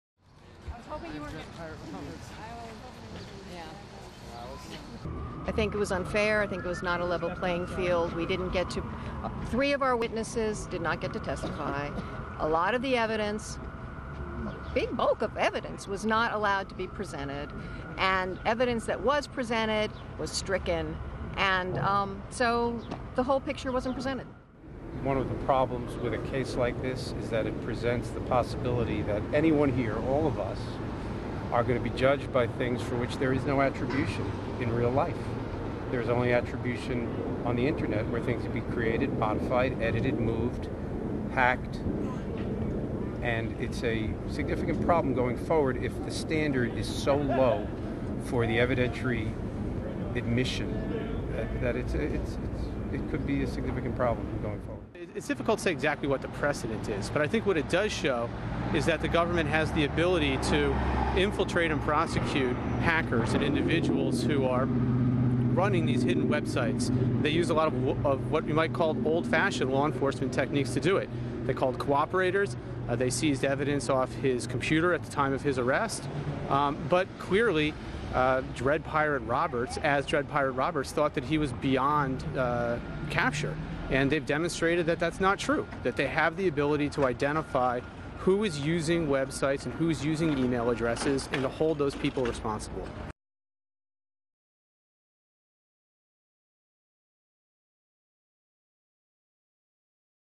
Ross Ulbricht's mother reacts to the conviction of her son - Daily Mail
Ross William Ulbricht was convicted of seven drug and conspiracy counts. Prosecutors said Ulbricht enabled more than 1 million drug deals on Silk Road and earned about $18 million in bitcoins. His sentencing in New York is scheduled for May 15, and some of the charges carry a maximum of life in prison.